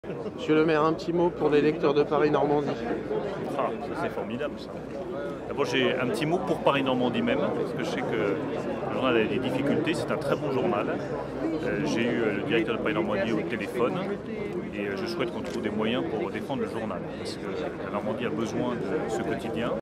Bruno Le Maire soutient Paris Normandie
Bruno Le Maire soutient Paris Normandie en dépôt de bilan au salon de l'agriculture